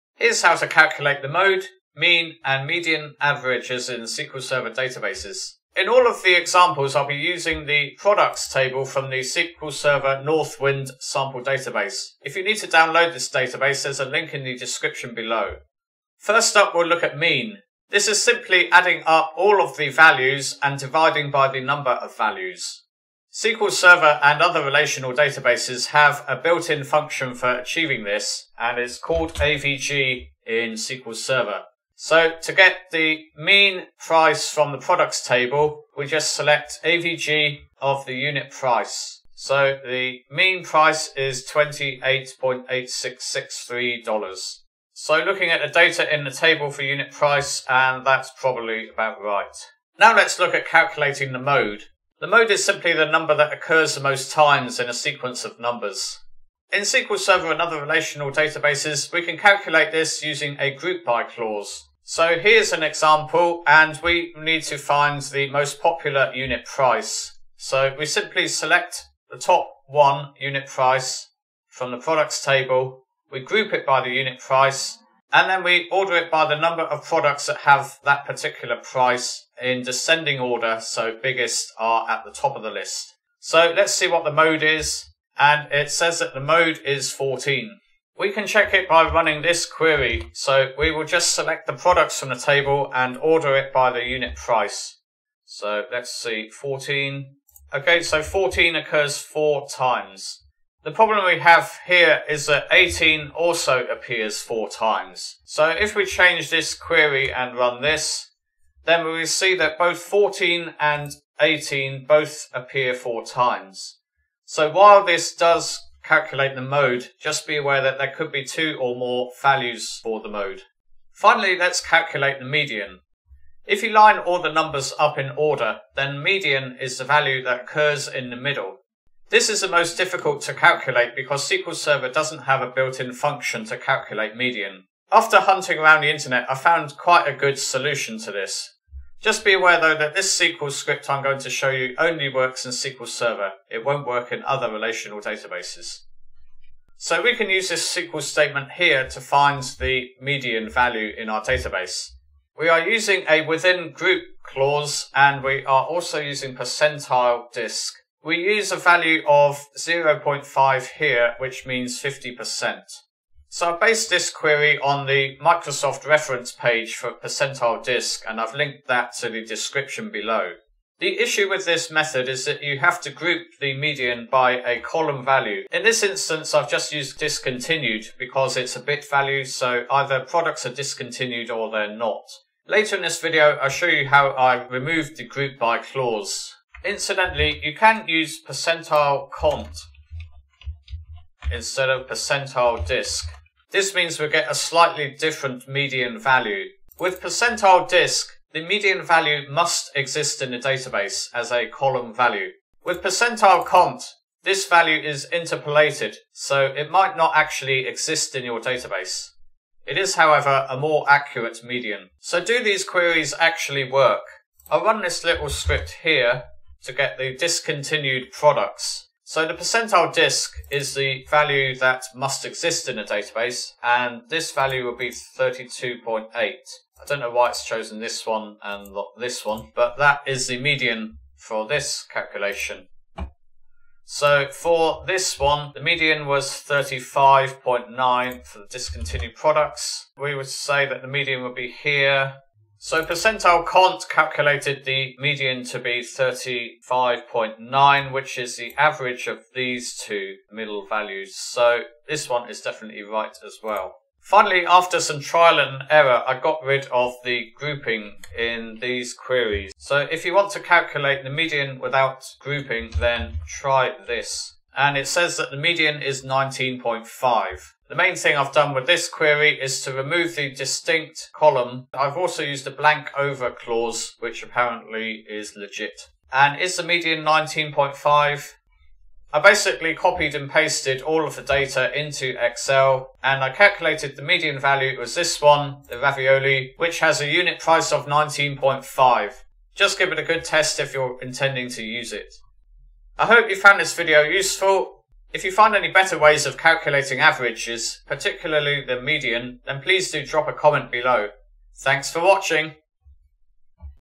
Calculating Averages in SQL Server (Mode, Mean, Median)
Here's how to calculate the mode, mean and median averages in SQL Server databases.

00:00 - Introduction
00:16 - Mean
00:50 - Mode
02:00 - Median
03:08 - PERCENTILE_CONT vs PERCENTILE_DISC
04:14 - Median without GROUP BY

The MEAN is the sum of all of the values divided by the total number of values. This is the easiest to calculate in SQL because we can simply use the AVG function:

SELECT AVG(UnitPrice) AS 'Mean Price' FROM Products

Most relational databases have an AVG function, although it might also be called AVERAGE.

The MODE is the most common value in a series of numbers. We can determine this with a GROUP BY clause. This will also work in MySQL and other relational databases.

SELECT TOP 1 UnitPrice
FROM Products
GROUP BY UnitPrice
ORDER BY COUNT(UnitPrice) DESC

Just be aware that in Northwind there are two product prices that appear the most times, so it's up to you what you want to do if that is the case with your database.

Finally the MEDIAN (middle value) is the most difficult to calculate because SQL Server has no built in function for calculating the median value. We can however make use of PERCENTILE_CONT or PERCENTILE_DISC using a query like this:

SELECT DISTINCT(Discontinued), PERCENTILE_DISC(0.5) WITHIN GROUP (ORDER BY UnitPrice)
OVER (PARTITION BY Discontinued) AS 'Median'
FROM Products

Normally these functions require a GROUP clause so in the above we're grouping by the 'Discontinued' column.

This appears to work without a GROUP clause:

SELECT TOP 1 PERCENTILE_disc(0.5) WITHIN GROUP (ORDER BY UnitPrice)
OVER () AS 'Median'
FROM Products

But make sure you test this thoroughly before using it for anything important.

Incidentally, you can use either PERCENTILE_CONT or PERCENTILE_DISC. With PERCENTILE_DISC the median value must exist in the database. PERCENTILE_CONT interpolates the result (so there may not be an equivalent row).

Both of these functions are specific to SQL Server - you'll need to find another way of calculating the median on MySQL and other relational databases.